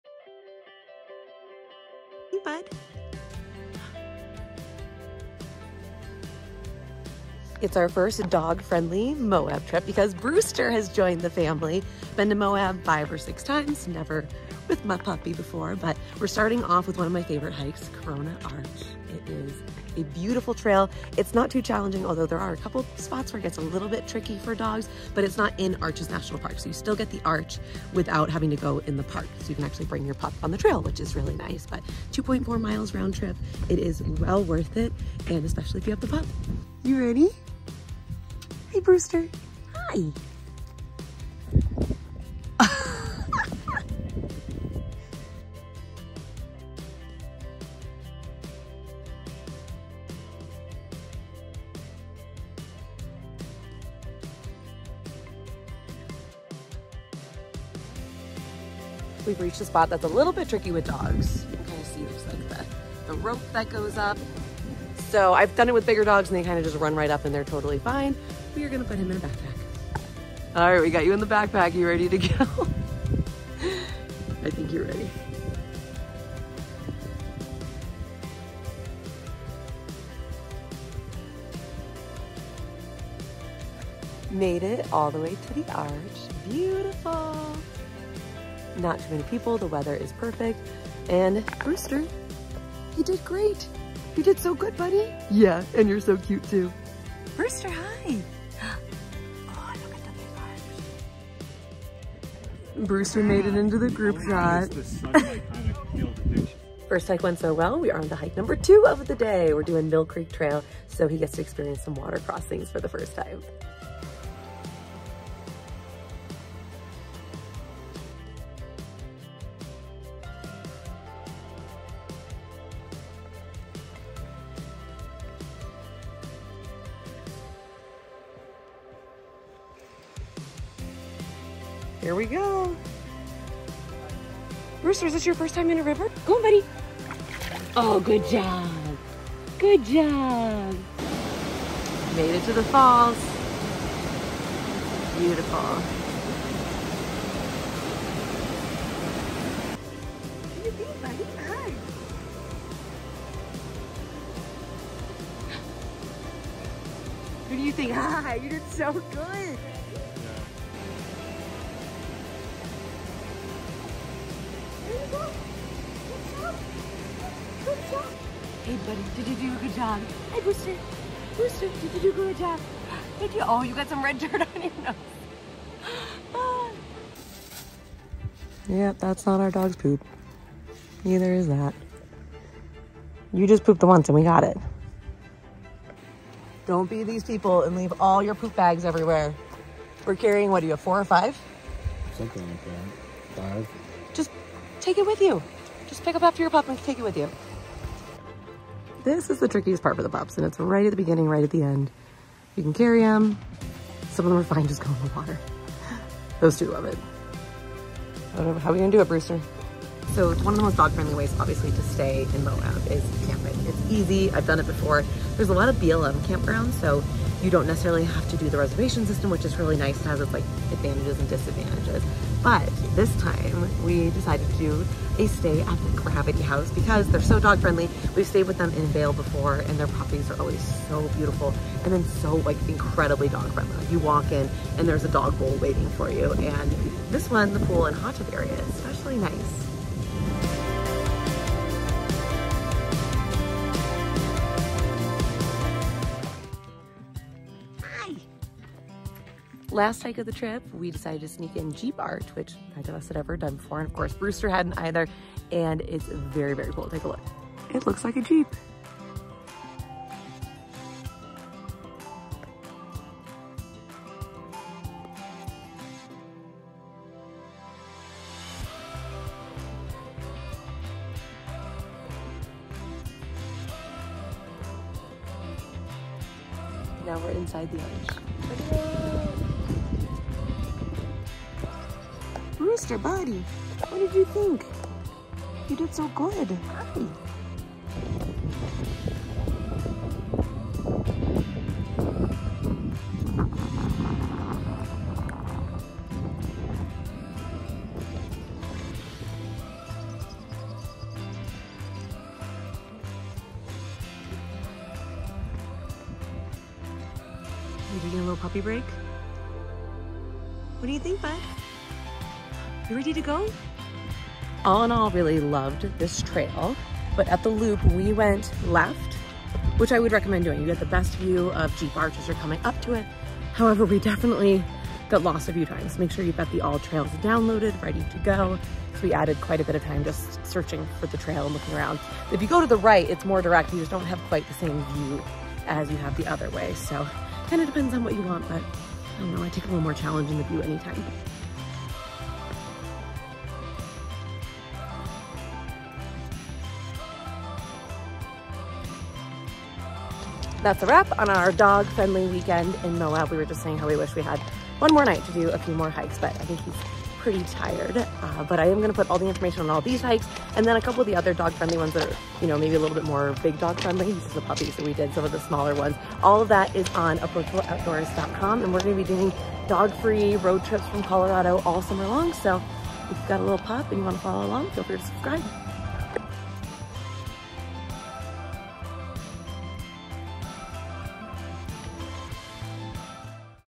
Dog-Friendly Weekend in Moab, Utah
With no dogs allowed on the trails in Arches and Canyonlands National Park, we found the perfect dog-friendly hikes in Moab, Utah! Brewster my our 5 month old pup - we think some sort of Heeler mix? We got the all clear from his vet to go hiking but still took it easy - he was carried over the steep parts and whenever he needed a break. Also, we noticed his harness kept coming loose on the first couple of hikes and went to get him a new one after (hence the wardrobe change)

Where we stayed:
Gravity Haus Moab - Super dog-friendly and the pool area is gorgeous!

Where we ate:
Thai Bella Moab
Sweet Cravings
Red Rocks Bakery
Moab Brewery
Spoke on Center

New to hiking? Start here!